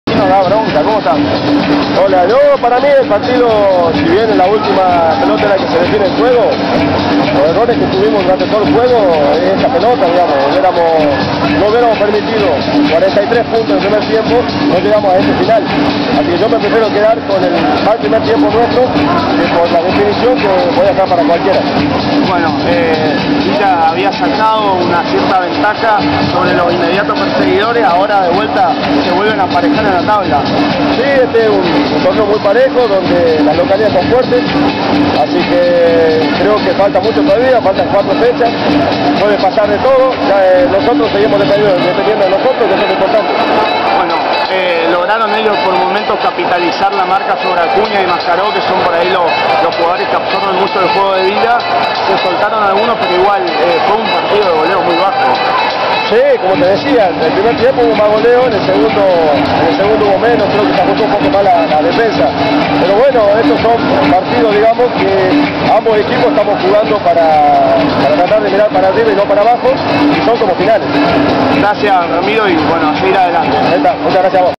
RAMIRO DIAZ CUELLO - ENTRENADOR, VILLA SAN MARTIN (Rcia. Chaco)
También, luego del encuentro por la 24ª fecha del TFB-Conf. NEA, el entrenador de Villa San Martín, Ramiro Díaz Cuello, reflexionó sobre la derrota sobre la hora ante Municipal Santa Rosa, y lo hizo en el aire de FM La Ruta y TSS.